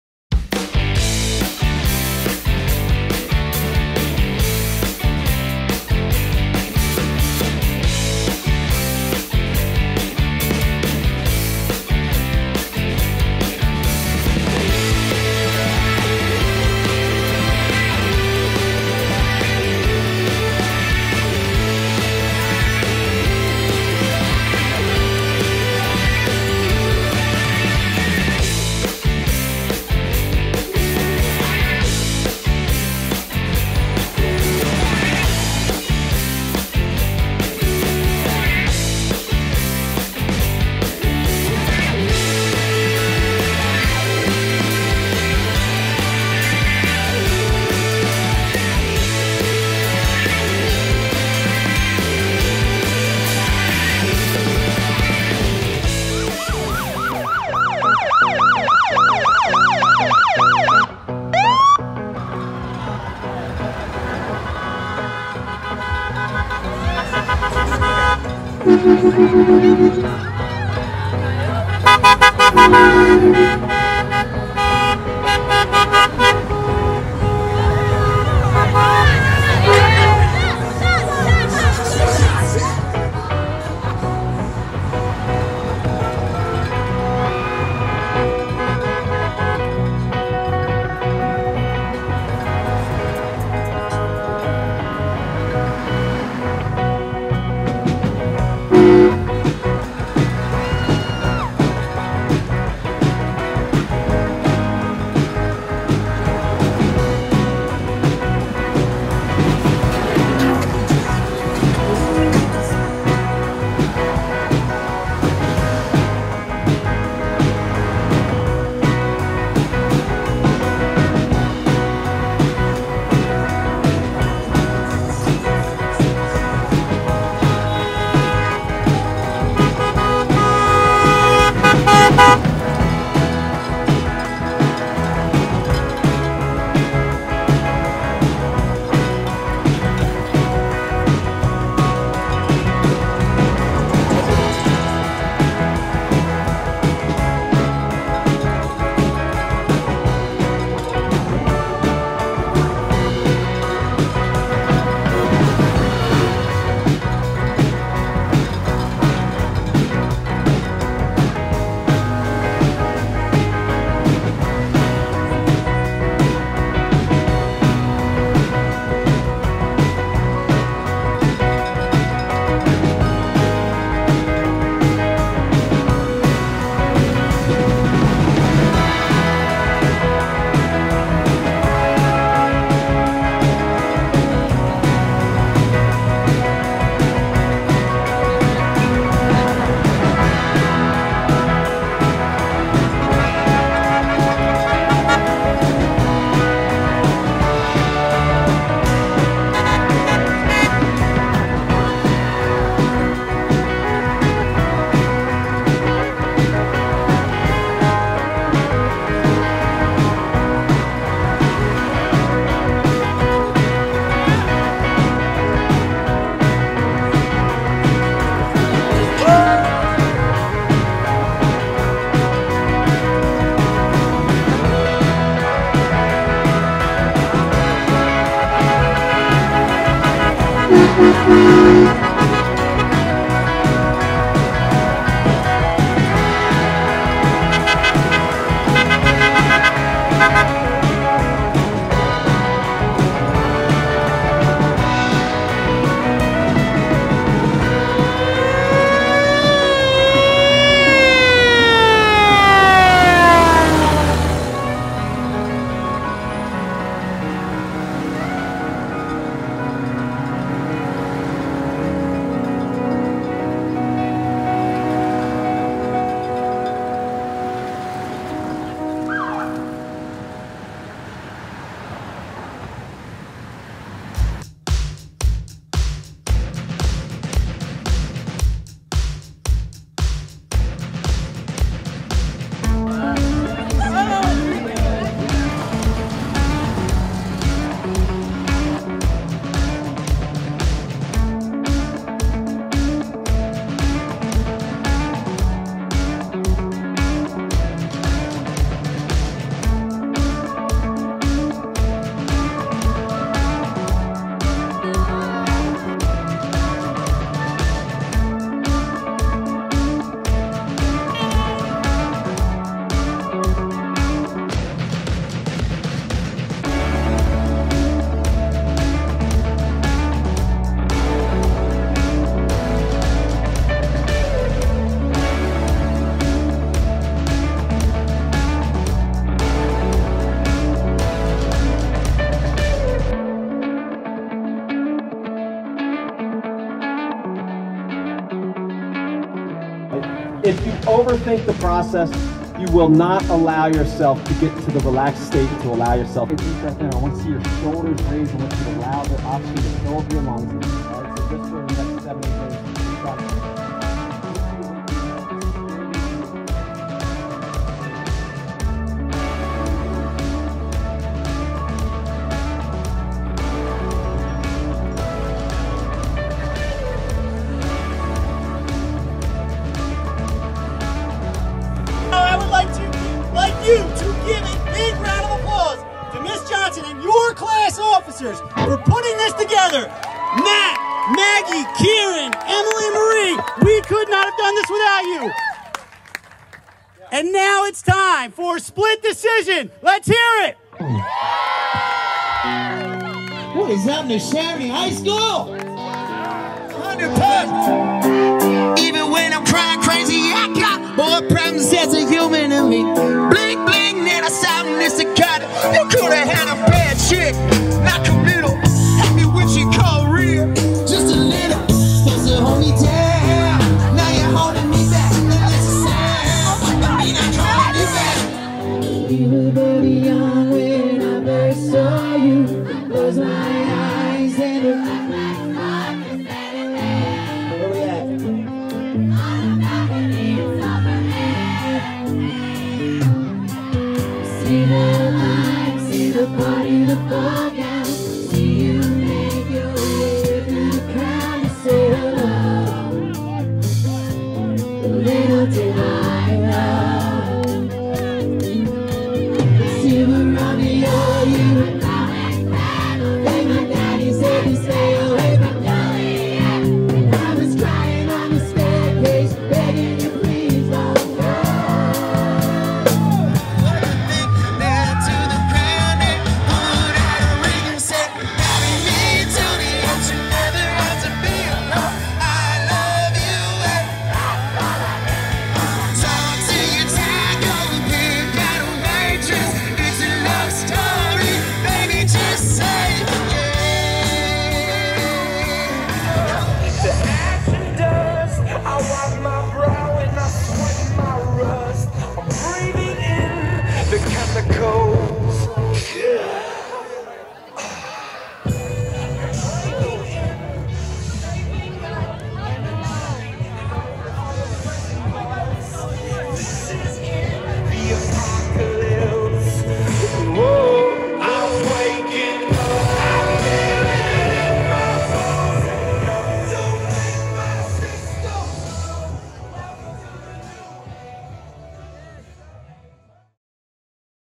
Neshaminy High School Car Parade and Senior Prom 2021
Parade: Maple Point to NHS through Langhorne; Prom at NHS